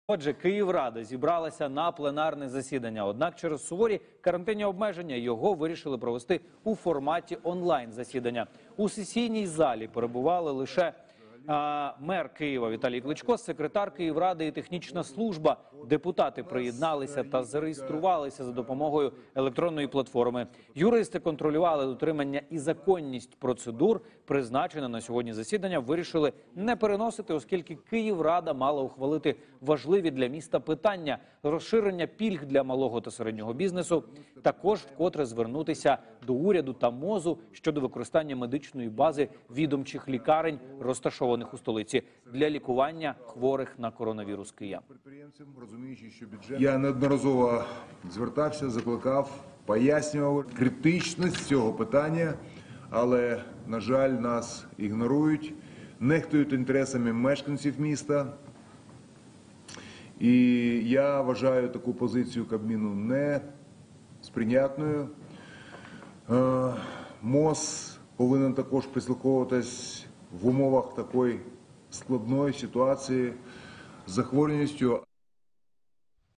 Київрада вперше проводить онлайн-засідання
Київрада вперше проводить онлайн-засідання, на якому розгляне низку питань протидії пандемії коронавірусу і підтримки бізнесу, повідомив міський голова столиці Віталій Кличко.